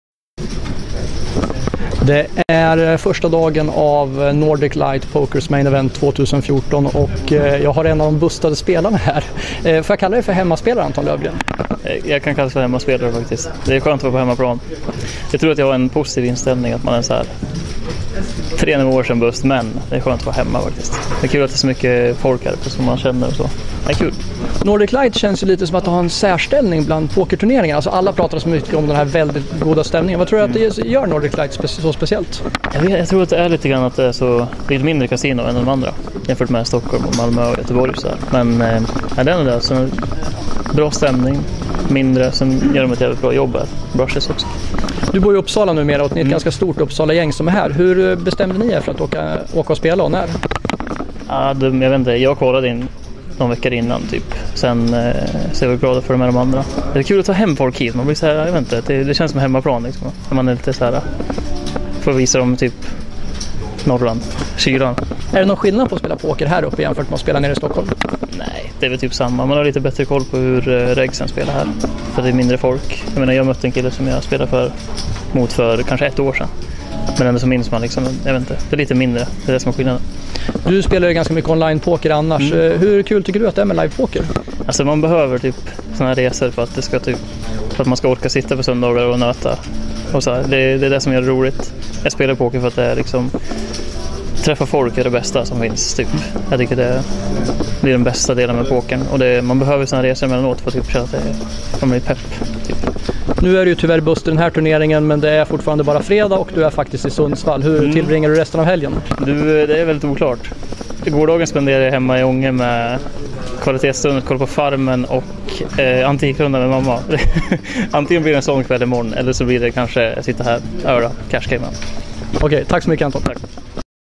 Anton Löfgren, Nordic Light 2014
Pokerresultat intervjuade Anton Löfgren under första dagen av main event i Nordic Light Poker 2014.